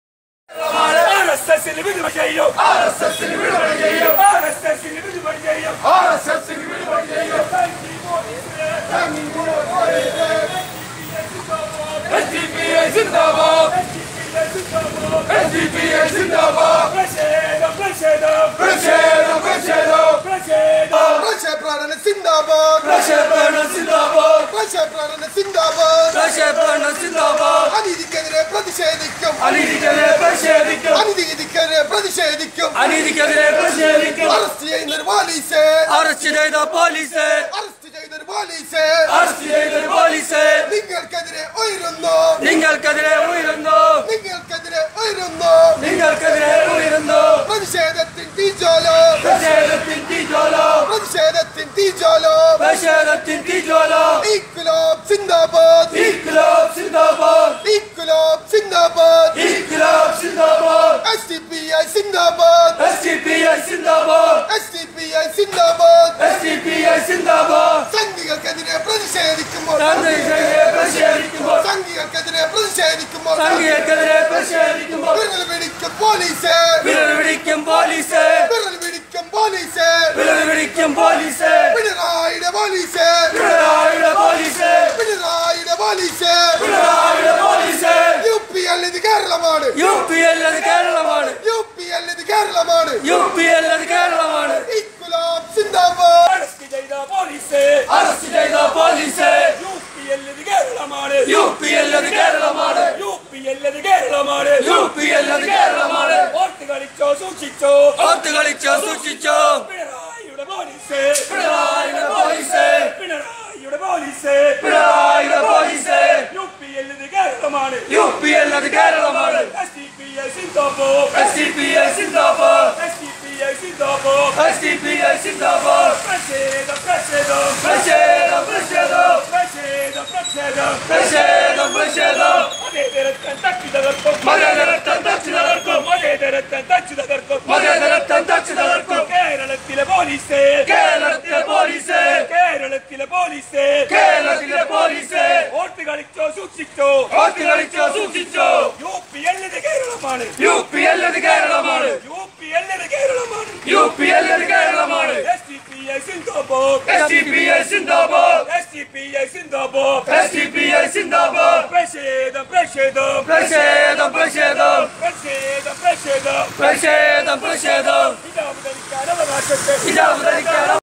SIO പ്രവർത്തകരെ അറസ്റ്റ് ചെയ്തു | SDPI പ്രതിഷേധപ്രകടനം | HIJAB ISSUE
ഹിജാബ് വിഷയത്തിൽ ഫോർട്ട് കൊച്ചി കടപ്പുറത്ത് പ്രതിഷേധിക്കാൻ എത്തിയ sio പ്രവർത്തകരെ അറസ്റ്റ് ചെയ്യതതിൽ പ്രതിഷോധിച്ച് SDPI മട്ടാഞ്ചേരി സിറ്റി കമ്മിറ്റിയുടെ നേതൃത്വത്തിൽ നടന്ന പ്രകടനം